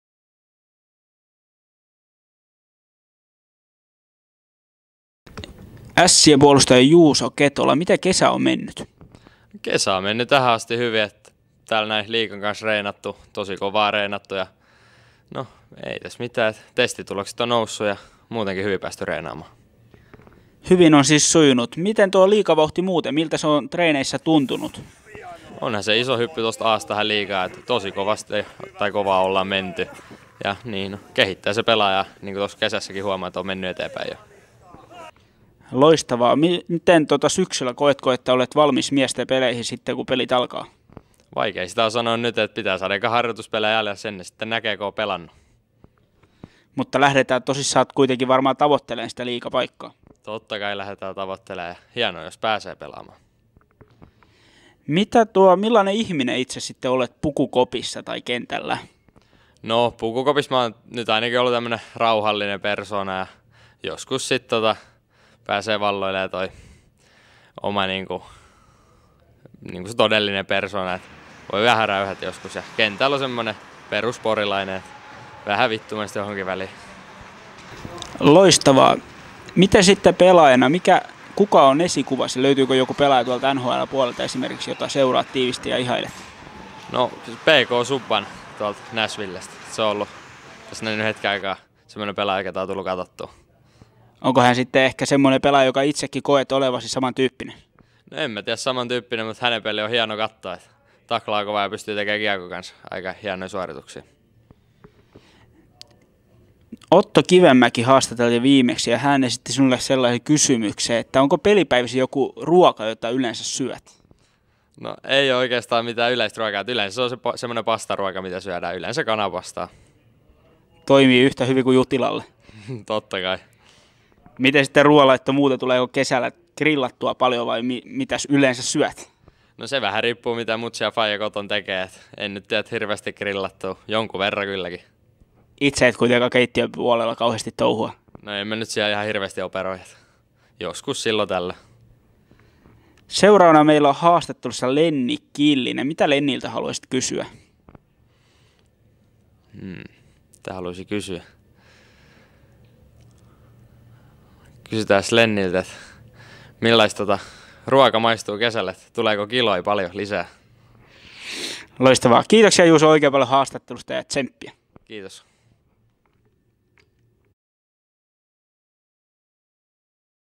Juuso Ketolan haastattelu
Nuori puolustaja ennakoi tulevaa kautta ja tavoitteitaan.